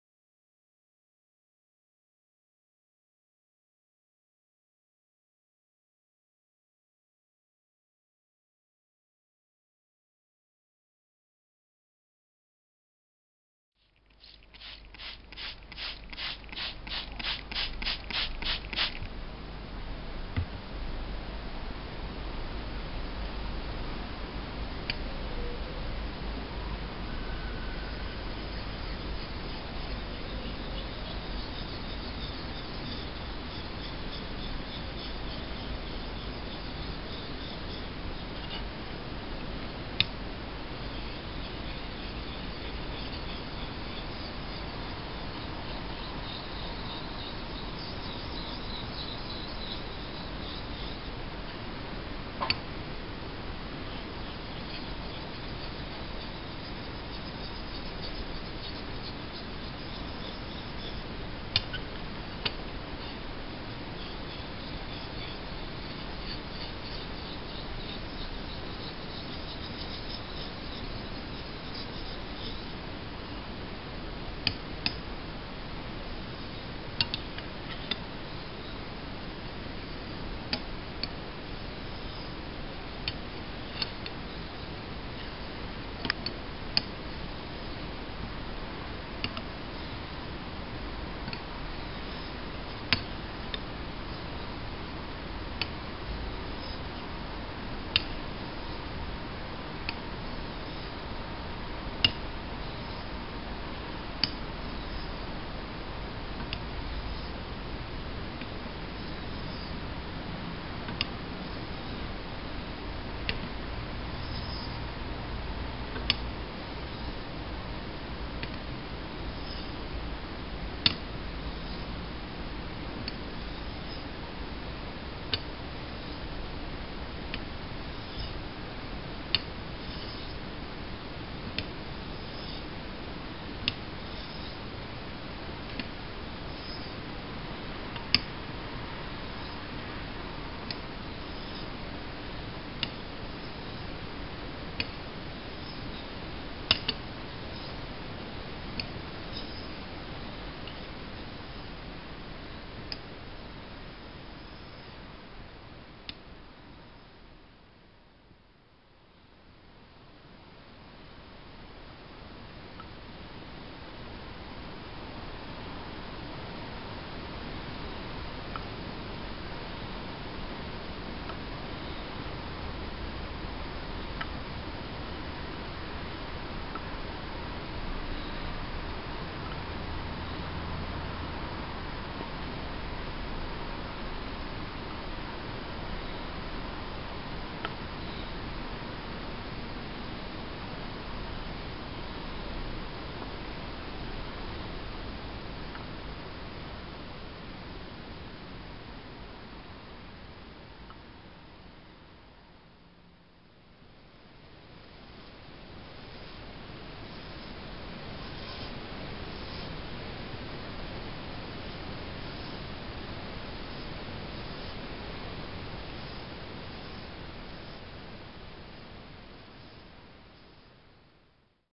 Echter Wasser Abziehstein - thuringian sharpening stone - part 3
Quick razor honing after setting the bevel off camera with a soft arkansas /pretty laborious process/.

It is not a gorgeous yellow-green 12k ""Escher"" Thuringian - but it is right at the 8k mark - so no problem to be used for honing razors.